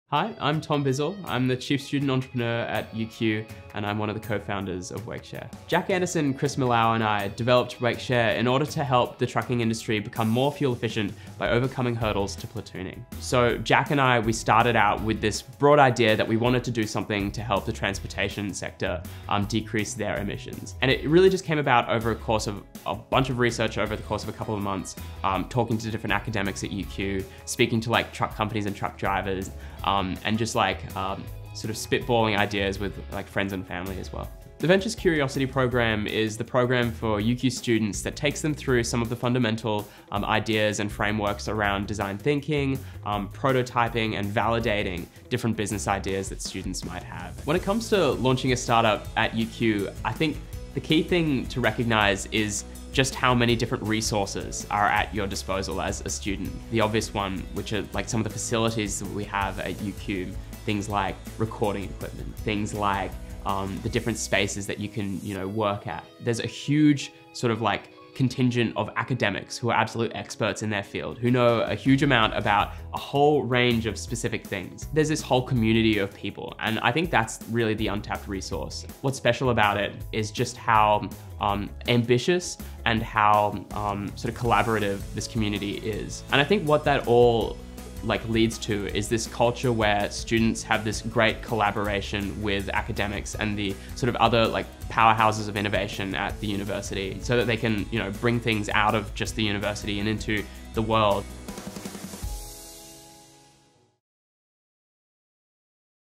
What's it really like to launch a startup at UQ?
At UQ, we’re continuously impressed by the entrepreneurial spirit and startup ideas that come out of our UQ Ventures community.
Tom Bizzell, a Bachelor of Commerce / Bachelor of Laws (Honours) student and UQ’s 2022 Chief Student Entrepreneur tells us about his startup Wakeshare and his experiences with UQ Ventures.